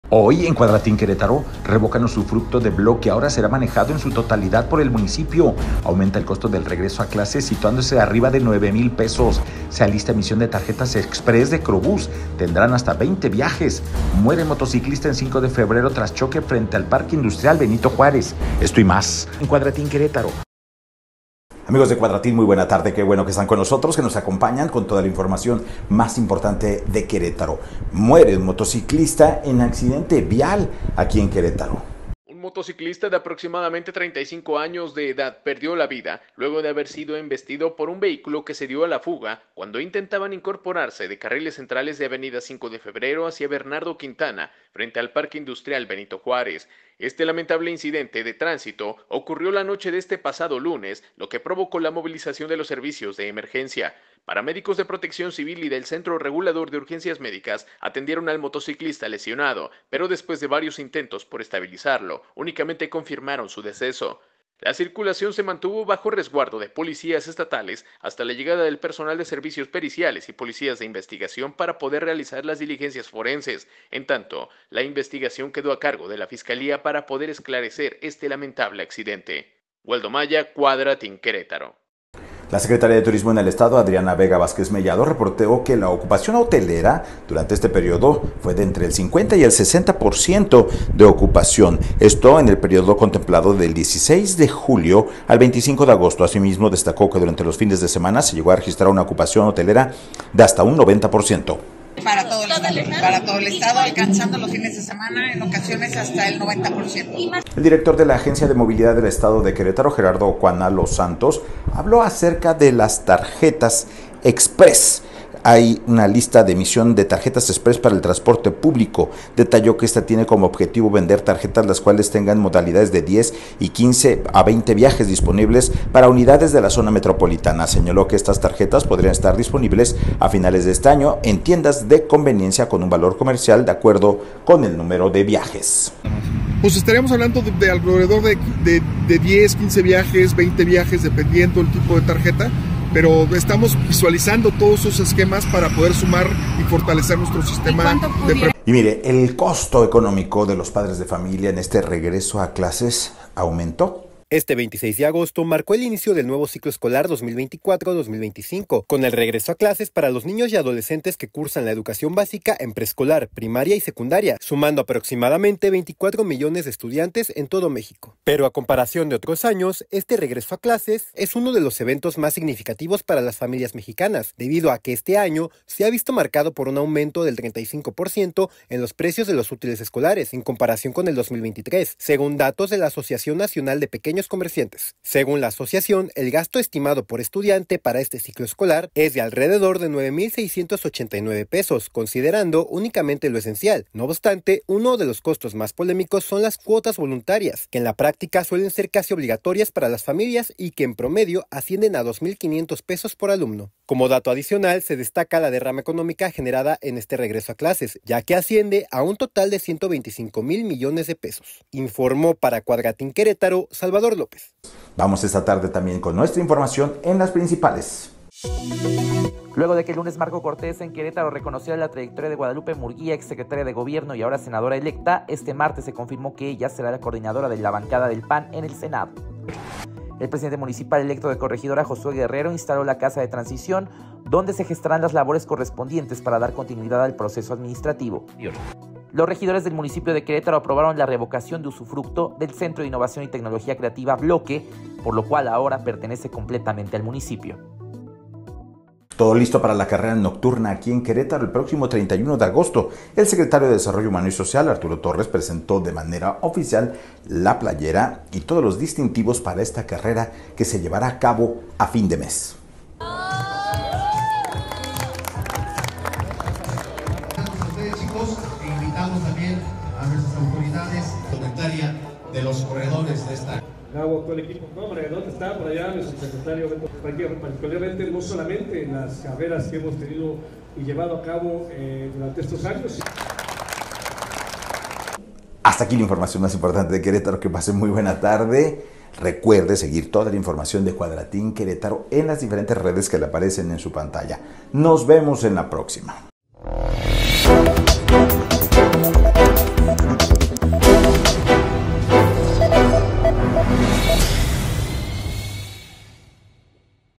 Las noticias con Adán Olvera 27 de agosto de 2024
👉🏻 REVOCAN USUFRUCTO DE BLOQUE, AHORA SERÁ MANEJADO EN SU TOTALIDAD POR EL MUNICIPIO DE QUERÉTARO

💸 AUMENTÓ EL COSTO DEL REGRESO A CLASES

🚍 SE ALISTA EMISIÓN DE TARJETAS EXPRESS DE QROBÚS

🚨 FALLECE MOTOCICLISTA EN 5 DE FEBRERO

📍Esto y más a las 19 horas por TODAS las plataformas digitales 🤳🏻🟠